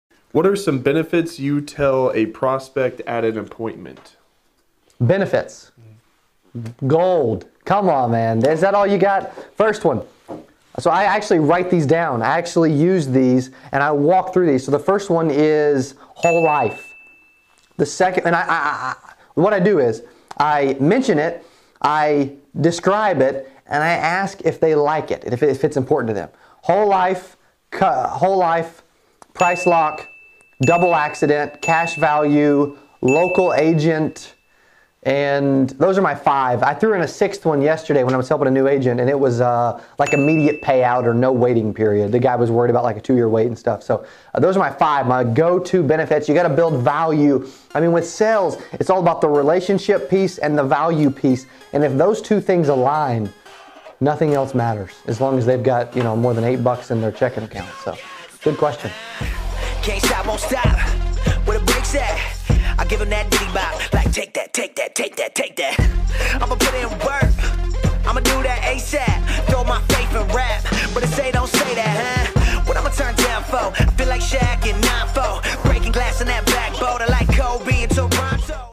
How Do You Add VALUE to a Sale? [Tips For Insurance Agents]
Giving a prospect 5 BENEFITS is an important step in the appointment process. 💼✍️

If you want to learn how to sell more persuasively, you need to understand what your prospects want. You can’t convince anyone to do anything if you don’t know what drives them. You have to figure out what their desires are.

Write down the benefit, describe to them what it is, and ask if it's important to them. You might surprised how well it works!